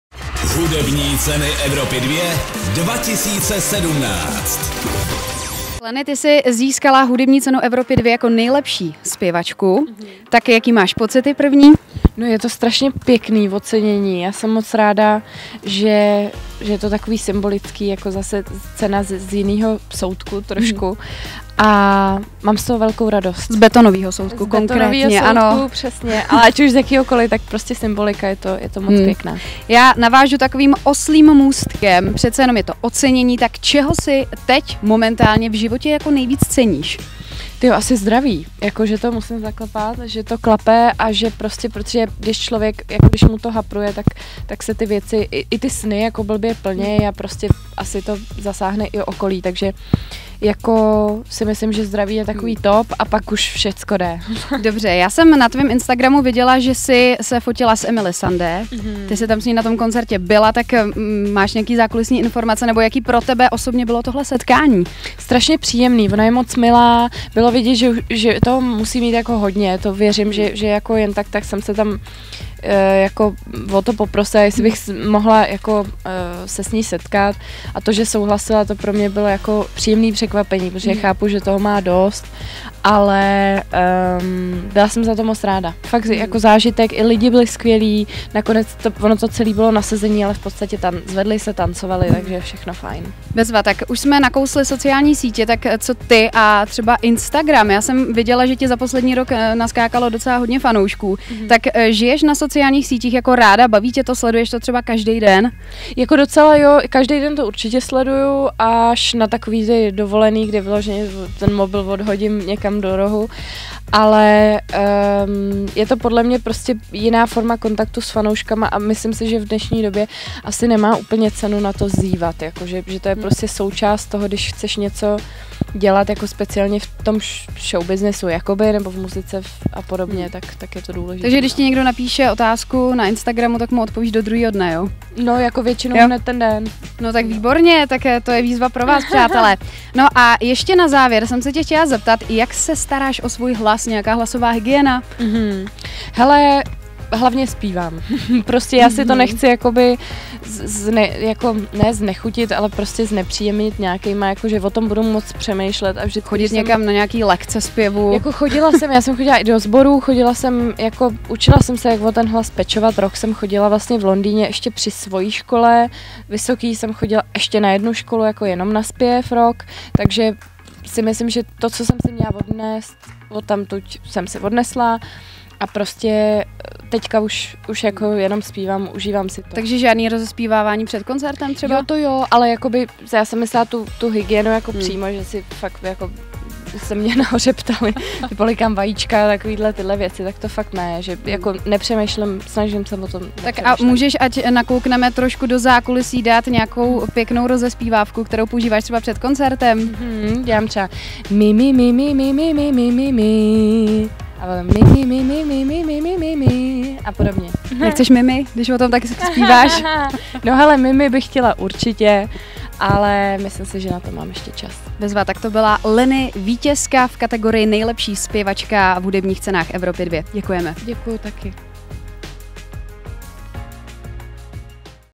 LENNY: Touží už zpěvačka roku po miminku?
ROZHOVOR S LENNY: Touží už zpěvačka roku po miminku?